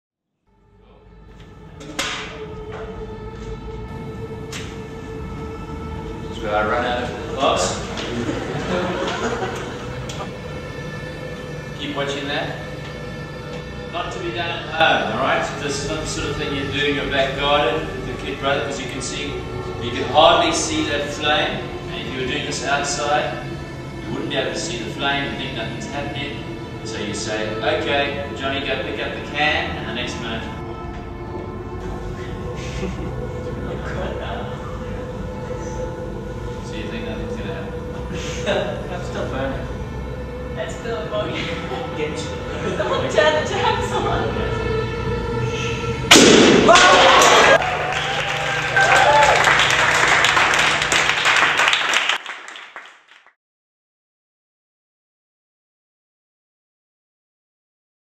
Exploding Can Experiment
A simple experiment to show the exothermic nature of some chemical reactions.